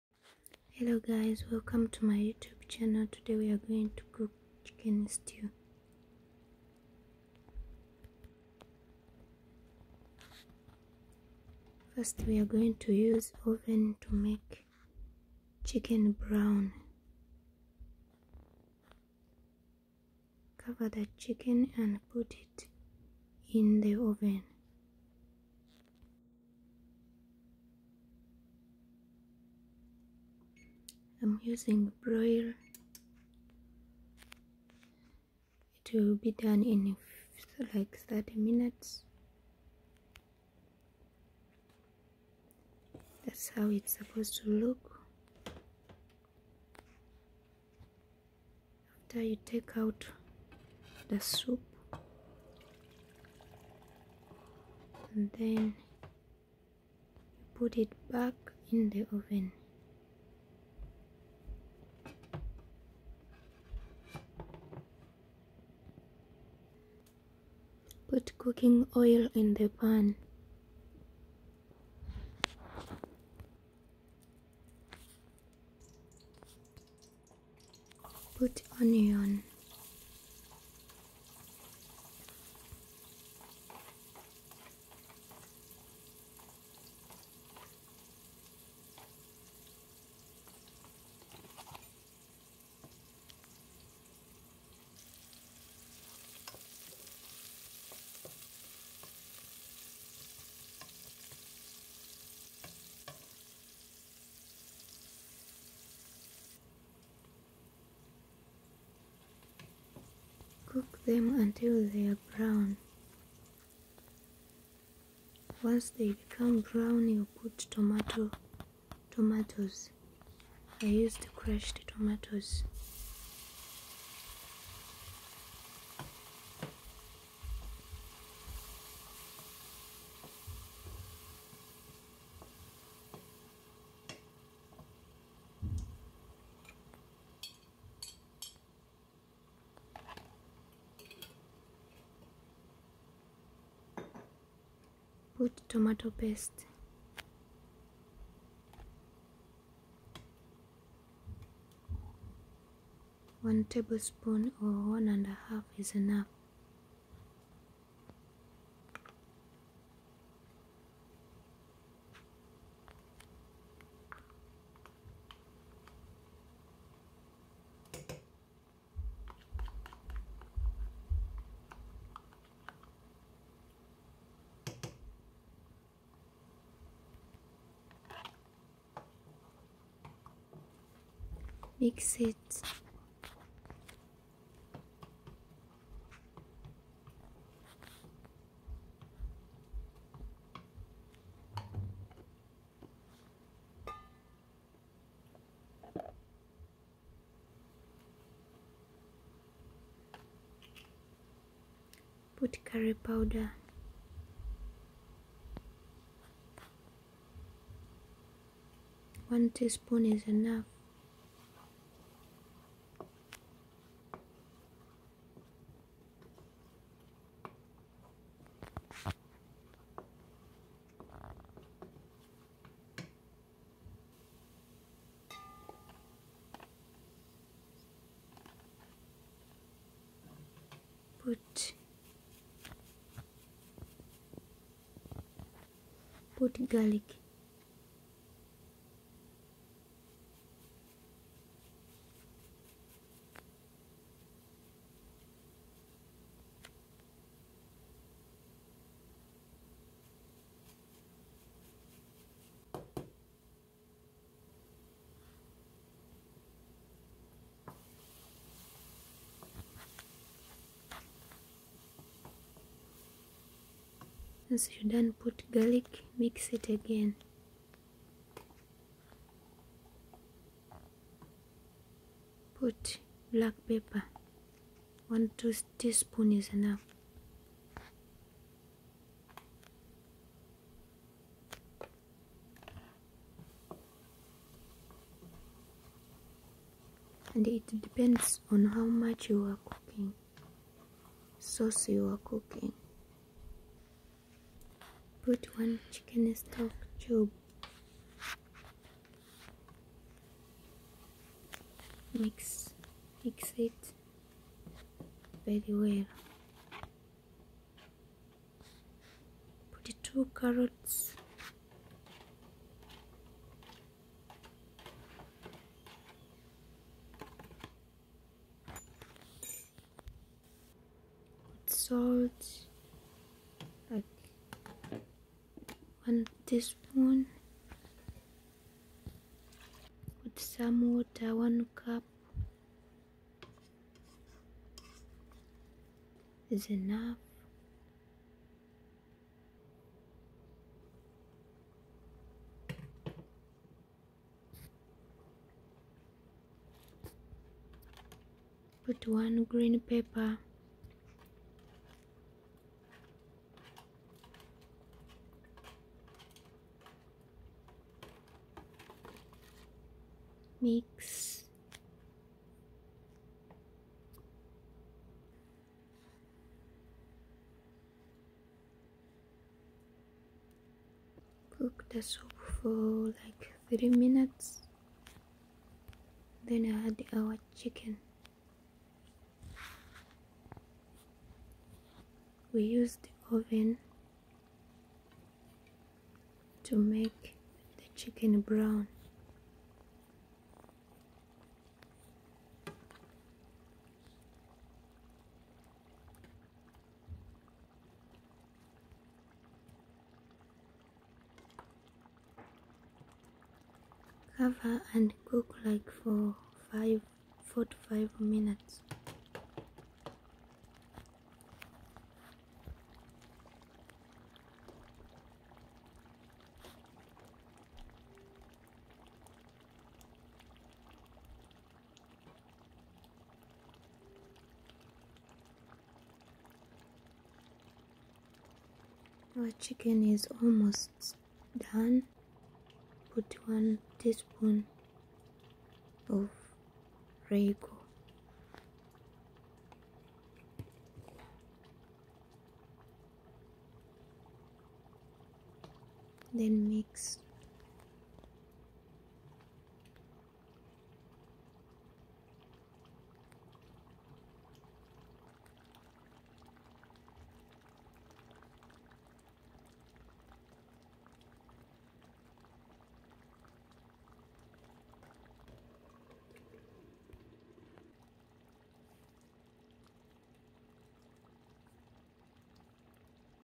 How to cook chicken stew
In this video am showing you how to cook chicken stew, Enjoy this tasty chicken recipe.

Ingredients
-Chicken 🍗
-Cooking oil
-1 Onion
-Crushed tomatoes
-1/2 tablespoons Tomato paste
- 1 tea spoon curry powder
- 5 cloves Garlic
-1 teaspoon black pepper
-Put 1 Chicken stock cube
-2 carrots 🥕
-Salt
- 1 cup water
-1 green pepper
- 1 teaspoon Royco